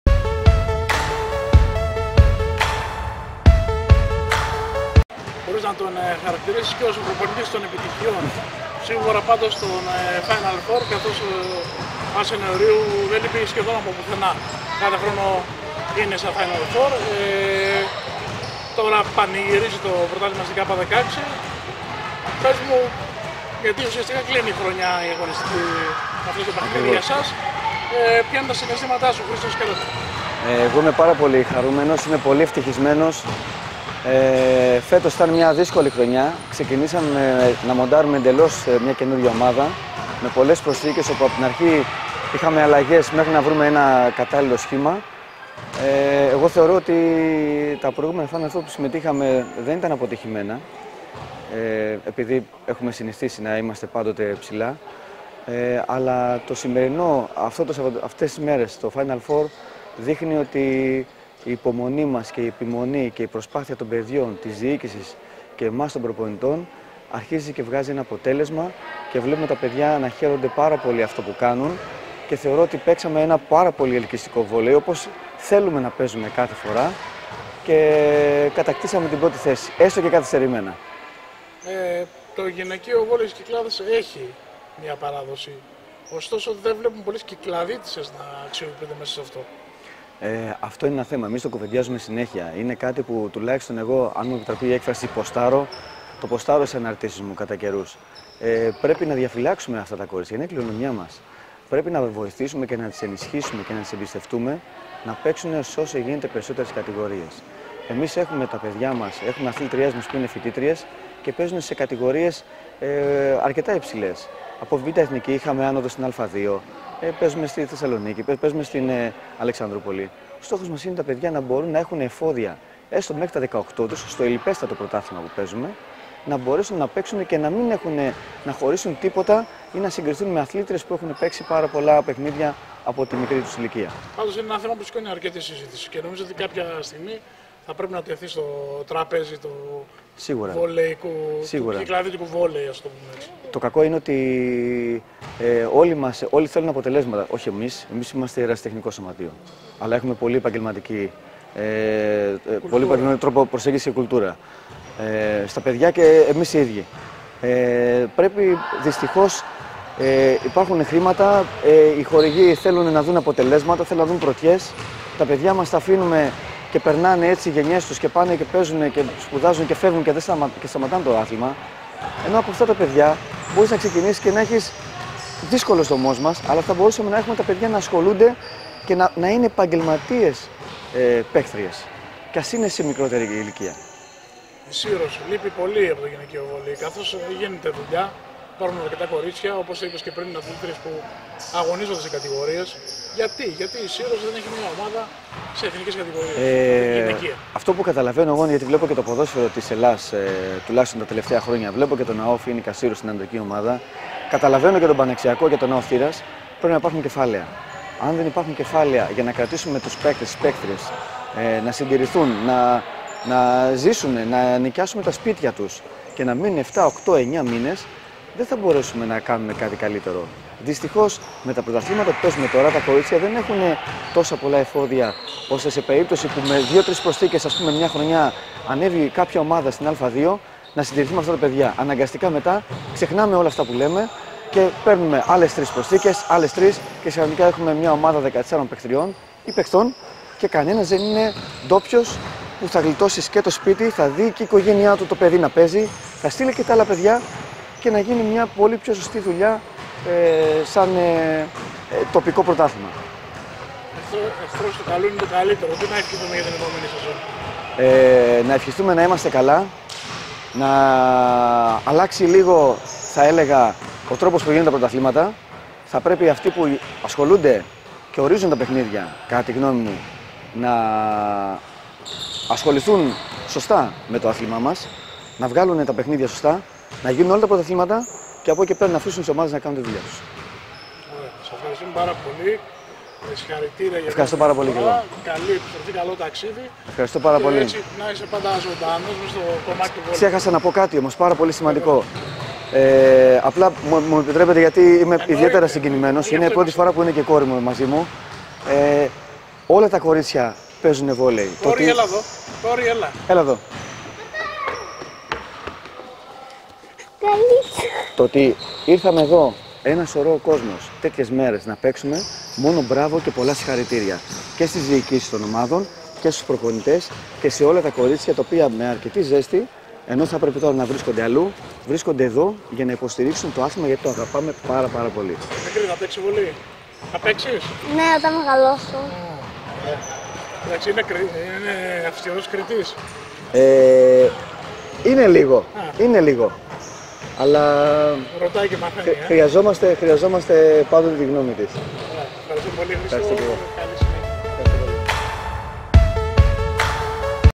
Χρήστος Σκρέτας: Είμαστε ερασιτεχνικό σωματείο με επαγγελματική κουλτούρα
Χαρούμενος και ευτυχισμένος με την κατάκτηση του πρωταθλήματος της Κ16 που ήταν το επιστέγασμα μίας δύσκολης χρονιάς, δήλωσε στο sportcyclades.gr o προπονητής του ΑΣΕ Νεωρίου, Χρήστος Σκρέτας.

Ο προπονητής των επιτυχιών του συριανού συλλόγου μίλησε για την επιτυχία της ομάδας του, αλλά και γενικότερα γαι το γυναικείο βόλεϊ στις Κυκλάδες.

Το καλύτερο κλείσιμο έγινε με το γλυκό φιλάκι που έλαβε από την κορούλα του.